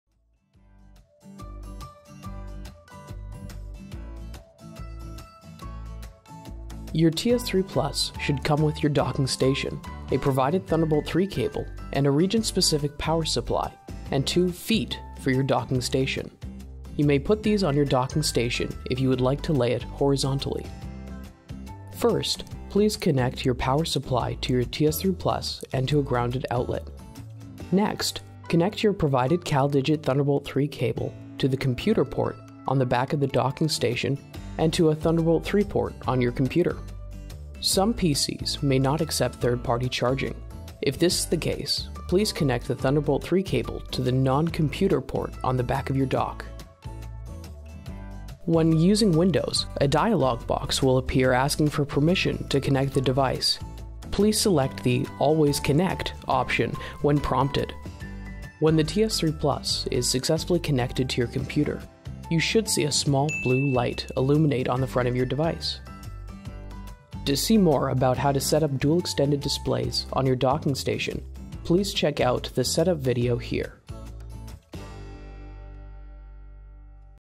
CalDigit Support: TS3 Plus Setup Guide
CalDigit TS3 Plus is the best Thunderbolt 3 docking station on the market. We have a simple setup guide you can follow and start a new experience with TS3 Plus.